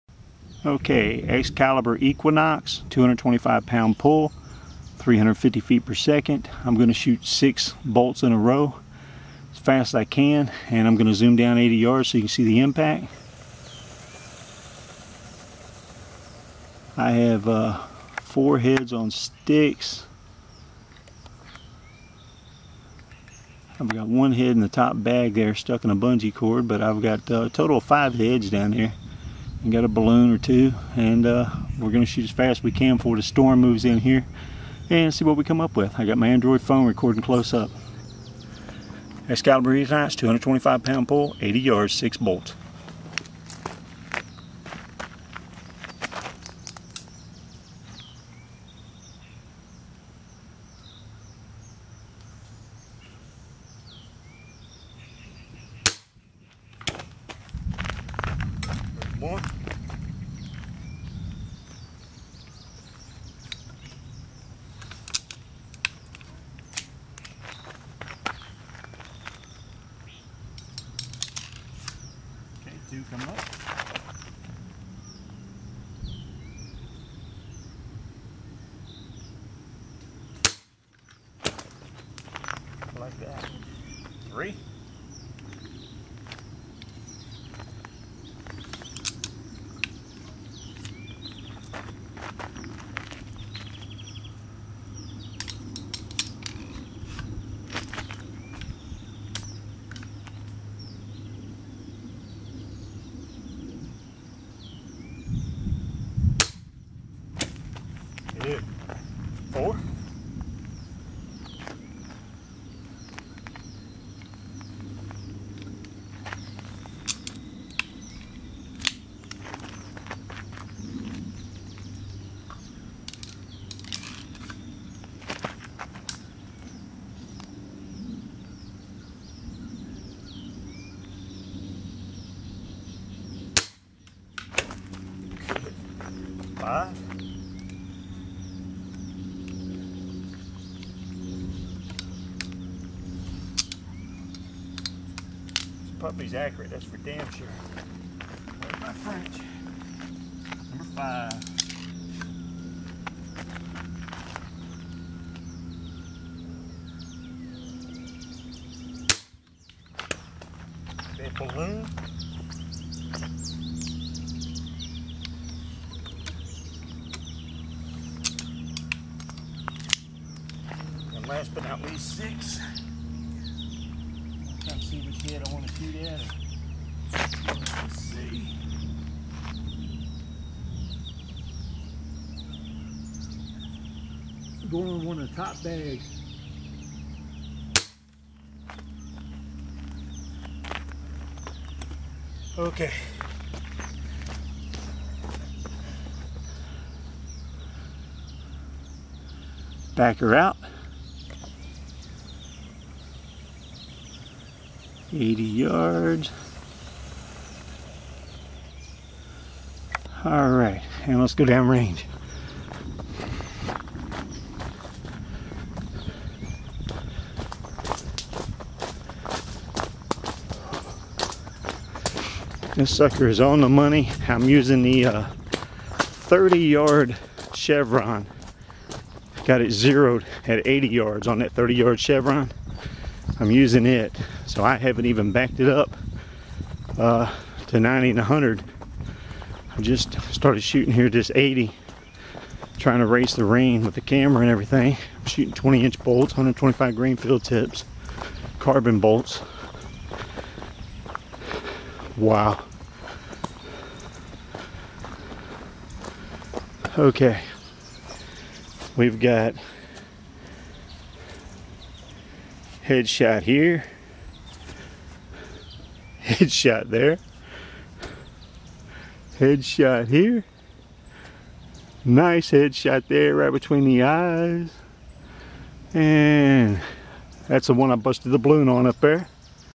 EXCALIBUR EQUINOX 225 LB PULL 80 YRDS 4 HEADSHOTS/
EXCALIBUR EQUINOX AT 80 YARDS AND 4 STYROFOAM HEADS ON STICKS WITH 2 BALLOONS. SHOT SIX IN A ROW. HIT 4 HEADSHOTS AND 2 BALLOONS WITH 5 BOLTS. THE 6 TH BOLT MISSING ENTIRE TARGET DISPLAY. NO IMPACT HE